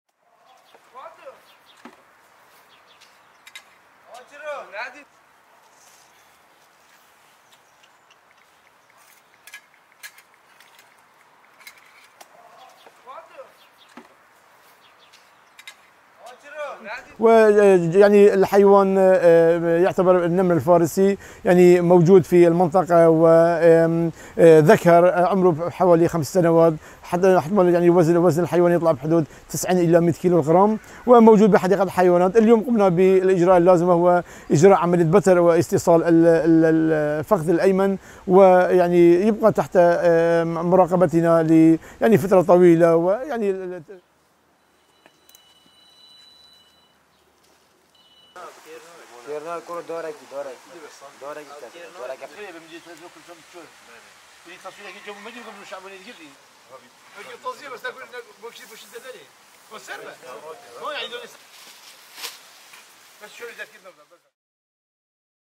Rare leopard captured in northern Iraq | AFP
An endangered leopard captured in Iraq's mountainous north had its hind leg amputated following a trap-inflicted wound. The Persian leopard, taken in a day earlier in Iraq's autonomous Kurdistan region near the border with Turkey, had injured two people, said Colonel Jamal Saado, head of the environmental protection police in Dohuk province.